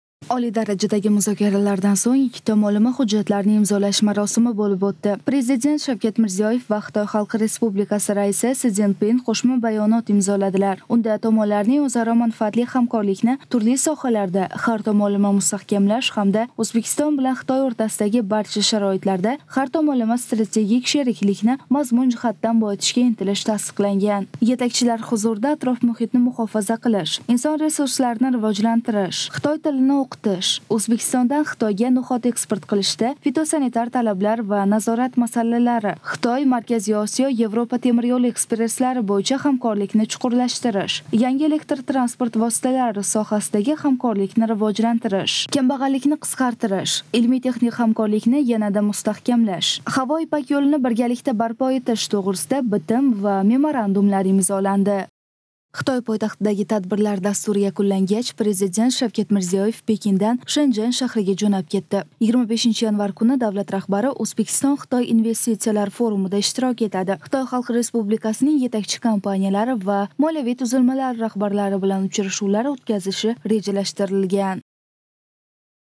O‘zbekiston va Xitoy o‘rtasida ikki tomonlama hujjatlarni imzolandi
Oliy darajadagi muzokaralardan so‘ng ikki tomonlama hujjatlarni imzolash marosimi bo‘lib o‘tdi. O‘zbekiston Respublikasi Prezidenti Shavkat Mirziyoyev va Xitoy Xalq Respublikasi Raisi Si Szinpin Qo‘shma bayonot imzoladilar. Unda tomonlarning o‘zaro manfaatli hamkorlikni turli sohalarda har tomonlama mustahkamlash hamda O‘zbekiston bilan Xitoy o‘rtasidagi barcha sharoitlarda har tomonlama strategik sheriklikni mazmun jihatidan boyitishga intilishi tasdiqlangan